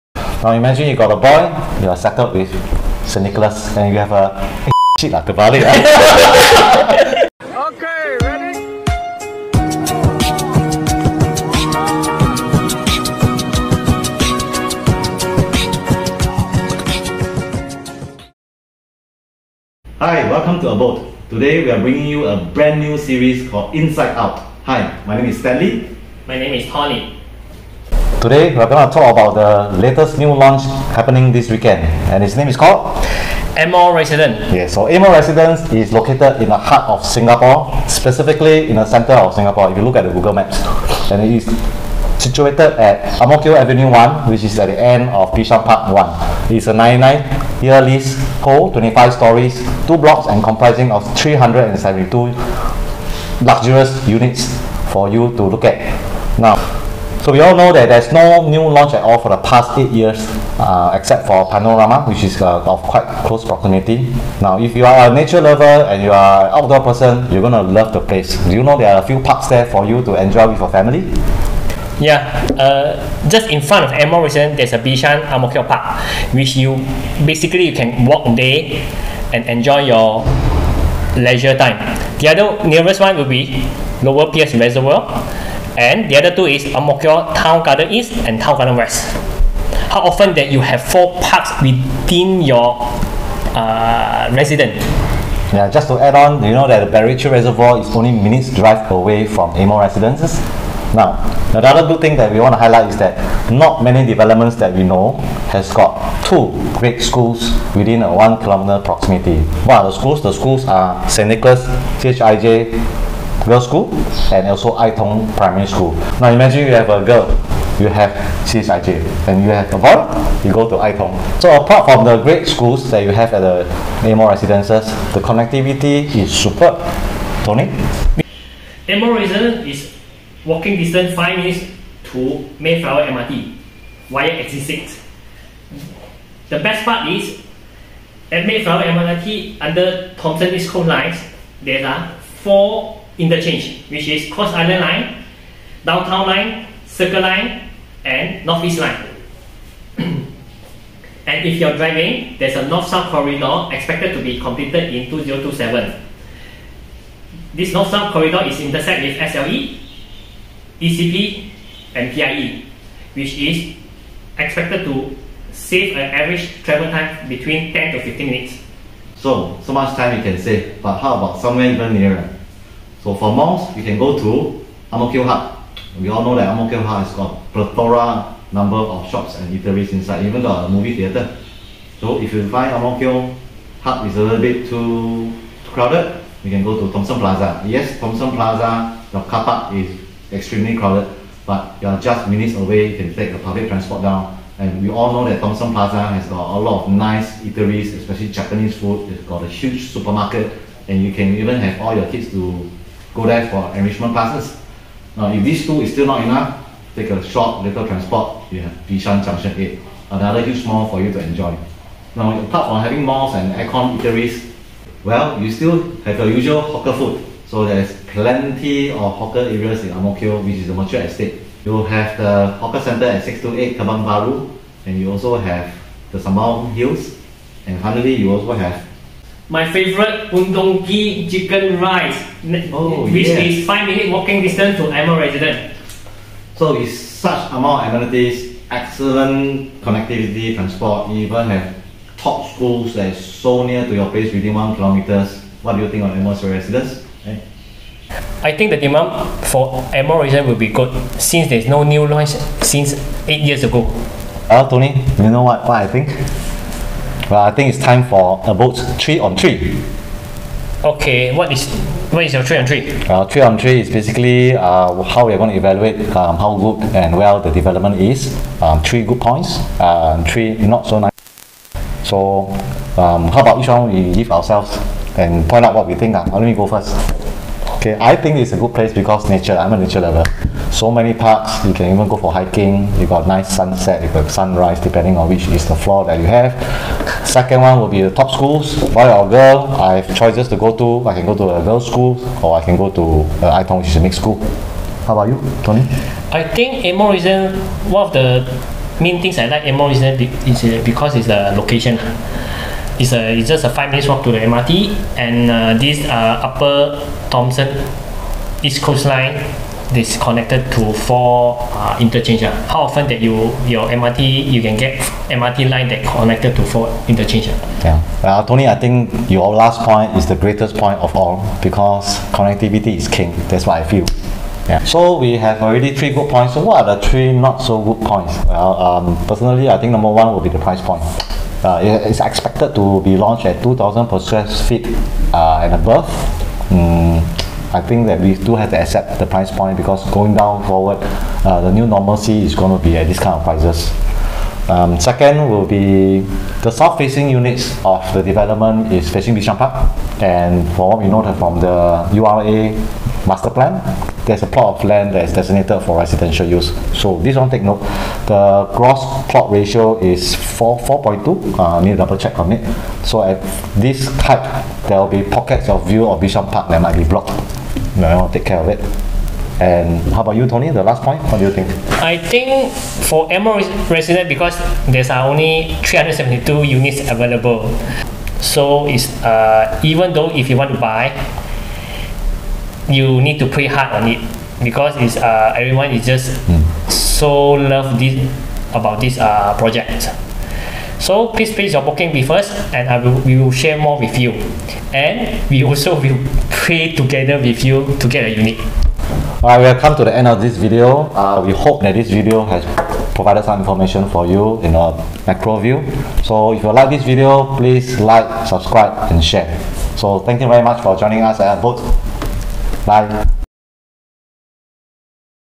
[AMO RESIDENCE] @ Ang Mo Kio Avenue 1
InSightOut Review of AMO Residence. Latest hottest new launch in Ang Mo Kio since 8 years ago!

Only 372 units, Call/whatsapp us/scan QR code to secure your slot, to avoid disappointment!
- 9th July’22: Preview
- 23rd July’22: BOOKING Day